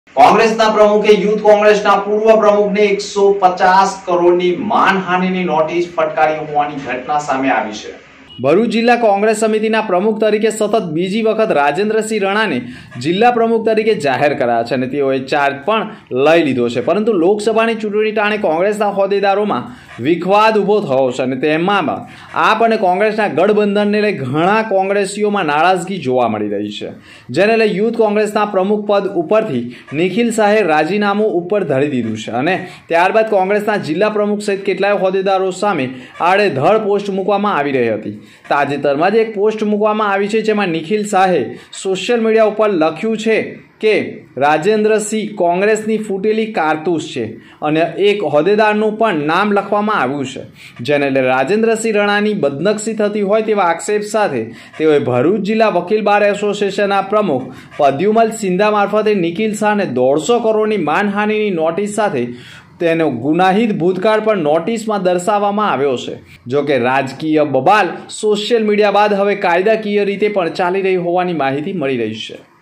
ભરૂચ જિલ્લા કોંગ્રેસ પ્રમુખે યુથ કોંગ્રેસના પૂર્વ પ્રમુખને રૂપિયા 150 કરોડની માનહાનિની નોટિસ ફટકારી
ભરૂચ જિલ્લા કોંગ્રેસ પ્રમુખે યુથ કોંગ્રેસના પૂર્વ પ્રમુખને રૂપિયા 1.50 કરોડની માનહાનિની નોટિસ ફટકારી..